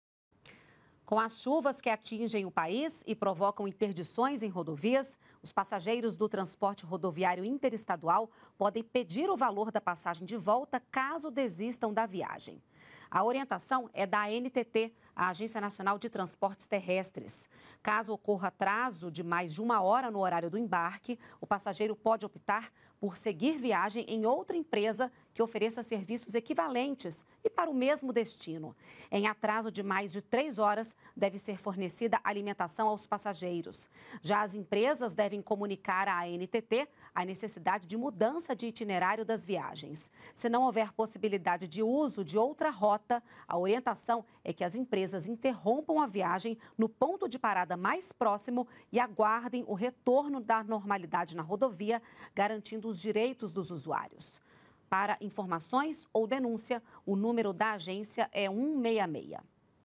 Transportes | Restituição de passagens em tempo chuvoso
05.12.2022 - Com as chuvas que atingem o país e provocam interdições em rodovias, os passageiros do transporte rodoviário interestadual podem pedir o valor da passagem de volta, caso desistam da viagem. Entenda com a jornalista Renata Corsini.